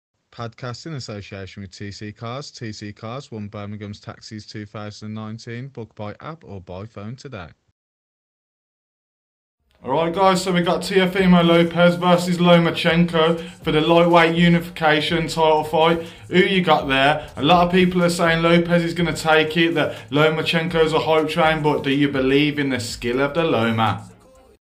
Vasiliy Lomachenko vs Teofimo Lopez in lightweight unification fight date confirmed!
Vasiliy Lomachenko vs Teofimo Lopez in lightweight unification fight date confirmed October 17th who wins?